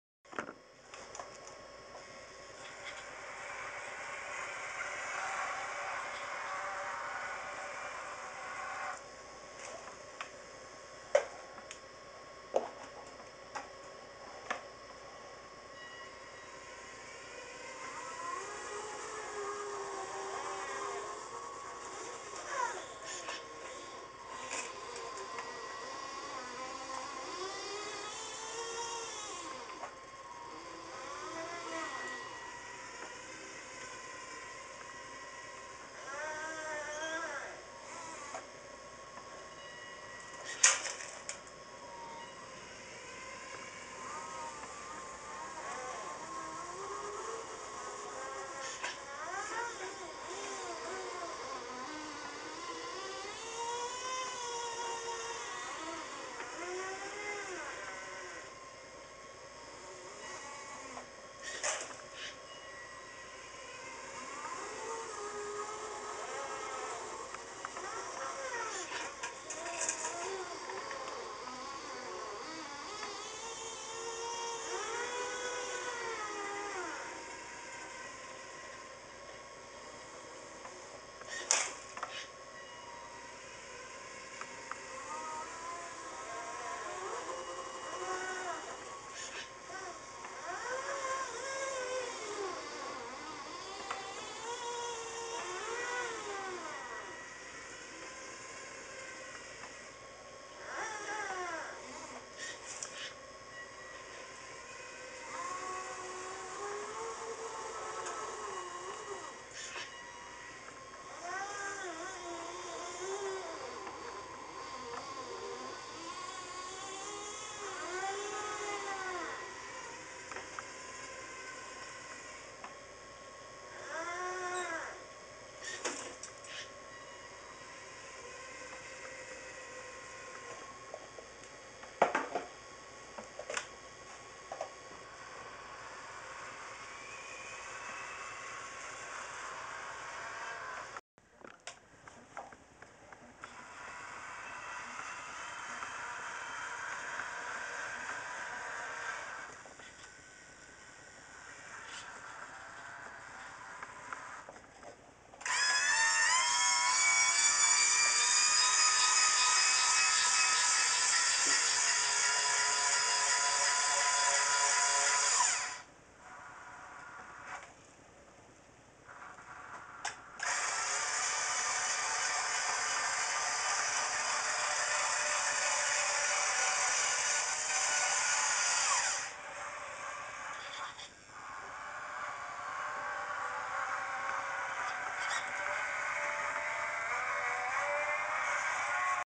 Marion Shovel Loading Cat Truck
This is my Marion 182M Shovel model loading my Cat 777D Truck model